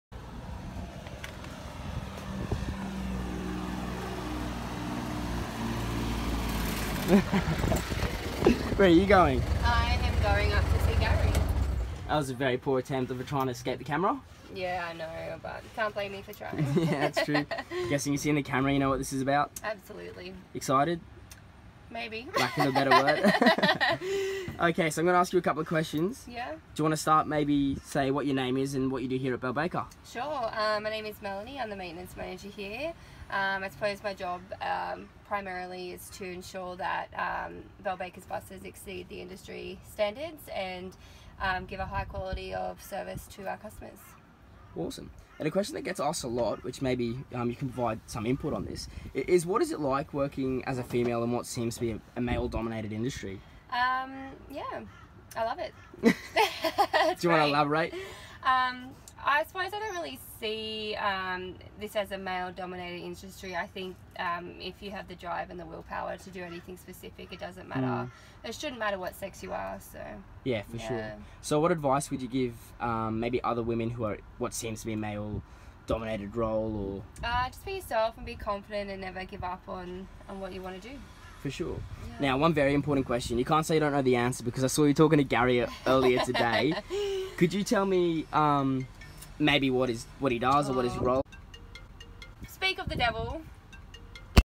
Belbaker Bus Charter (Blog Series) Episode 07 - Maintenance Manager
In this week’s episode of ‘Inside Belbaker’ we interrupt our Fleet Maintenance Manager, Mel, in her tracks to pay the infamous Gary a visit!

As the Maintenance Manager, Melanie has the valuable role of ensuring our coaches surpass our client’s expectations in every regard.

If you like what you see, don’t forget to give us a thumbs up or cheeky share!